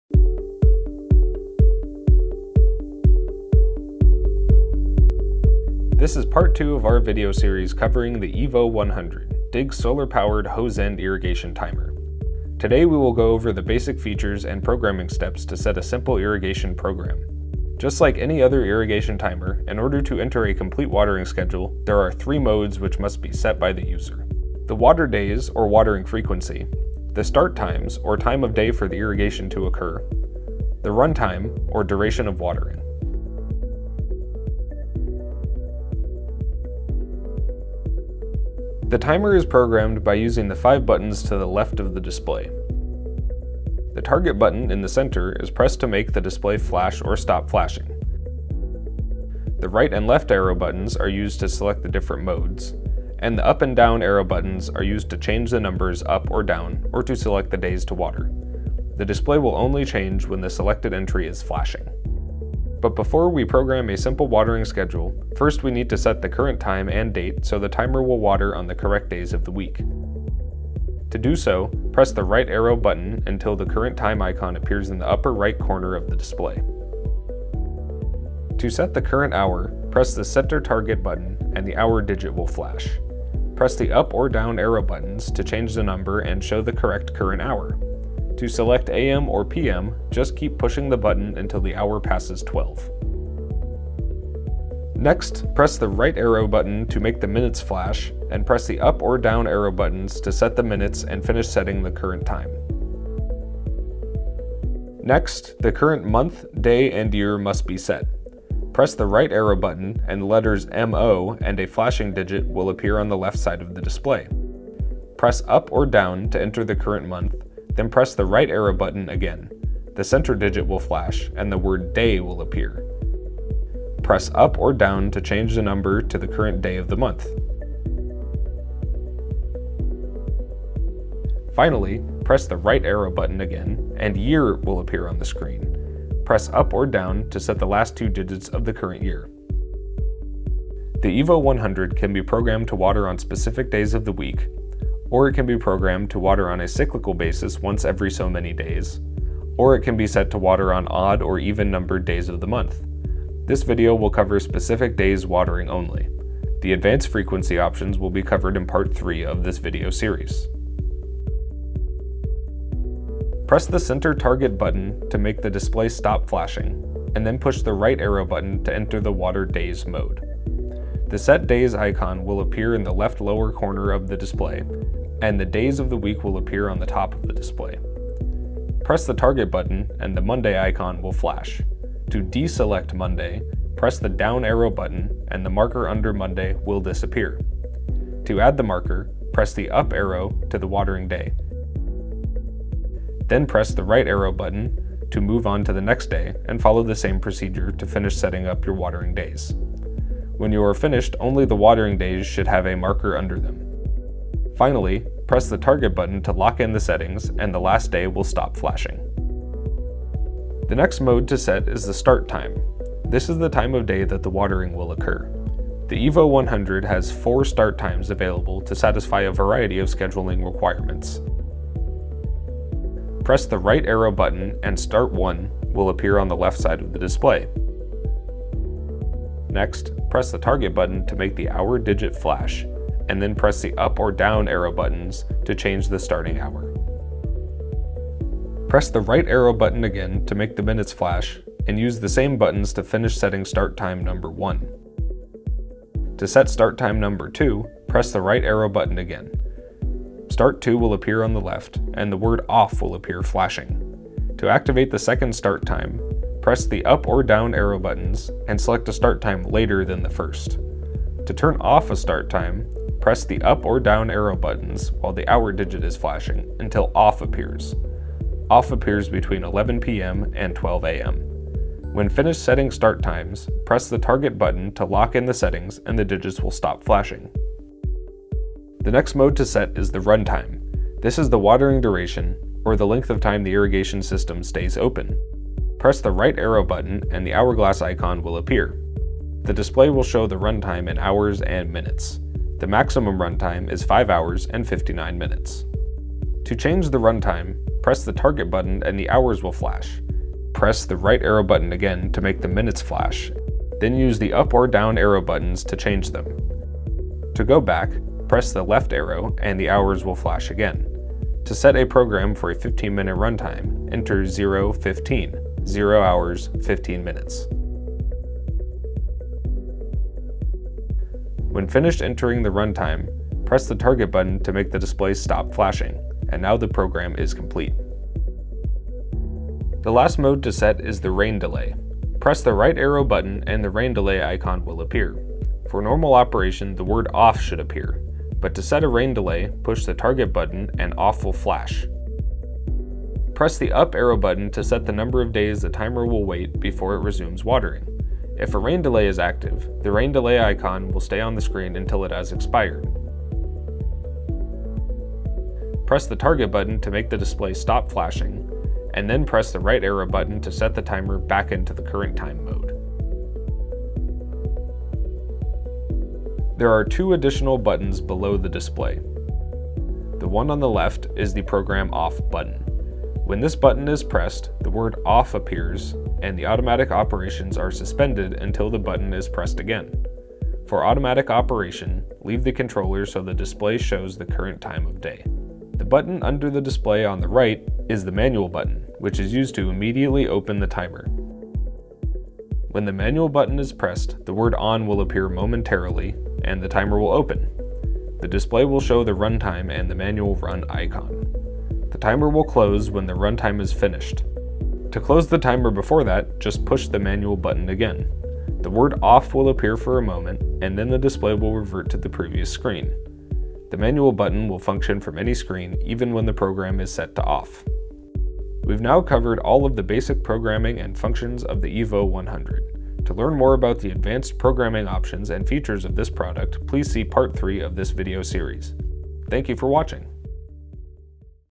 DIG EVO100 Solar Powered Hose End Timer (Tap timer) Part II: Basic Programming
This is Part Two of our video series on the EVO 100, DIG'S Solar Powered Hose End Irrigation Timer. In this video we cover Basic programming techniques.

To find out more info about DIG and the EVO100, visit our